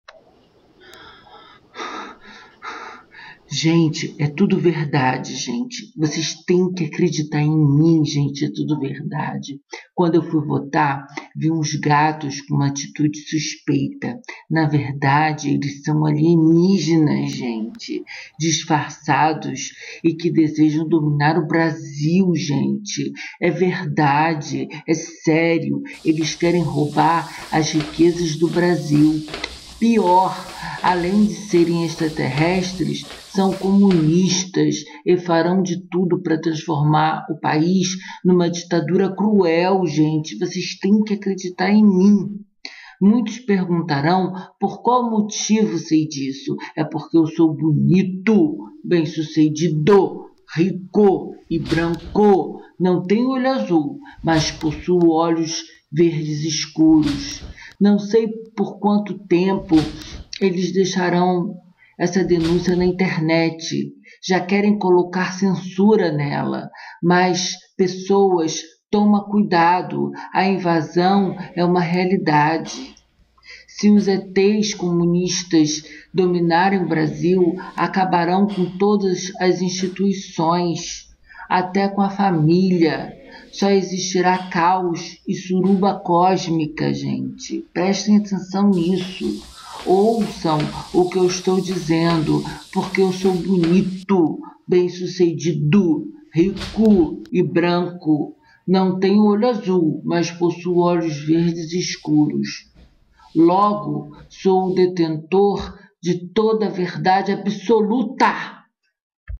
DENÚNCIA
Gente, é tudo verdade! Quando fui votar, vi uns gatos com uma atitude suspeita. Na verdade, eles são alienígenas disfarçados que desejam dominar o Brasil. É sério, eles querem roubar as riquezas do Brasil. Pior, além der serem extraterrestres são comunistas e farão de tudo para transformar o país numa ditadura cruel. Muitos perguntarão por qual motivo sei disto? É porque sou bonito, bem sucedido, rico e branco( não tenho olho azul, mas possuo olhos verdes escuros!). Não sei por quanto tempo, eles deixarão essa denúncia na internet, já querem colocar censura nela, mas, “pessoas” tomem cuidado, a invasão é uma realidade. Se os ETS comunistas dominarem o Brasil, acabarão com todas as instituições, até com a família. Só existirá caos e suruba cósmica. Ouçam o que estou dizendo!!! Porque sou bonito, bem sucedido, rico e branco( não tenho olho azul, mas possuo olhos verdes escuros!), logo, sou detentor da VERDADE ABSOLUTA.